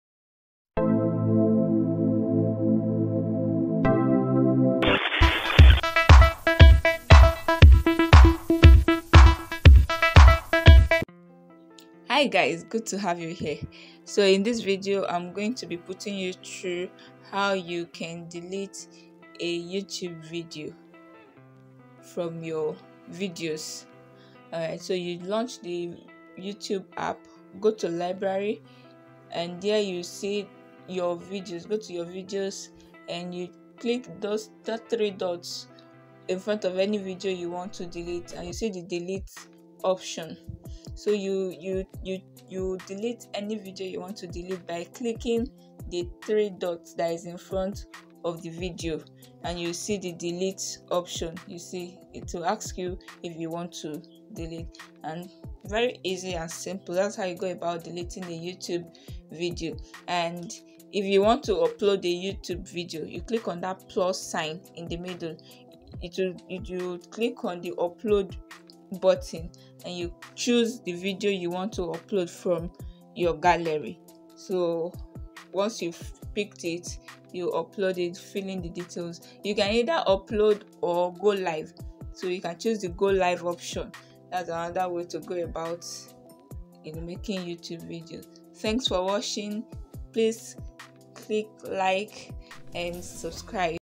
HOW TO DELETE A YOUTUBE VIDEO
Very short beginner friendly explanation of how to delete  or upload a youtube video.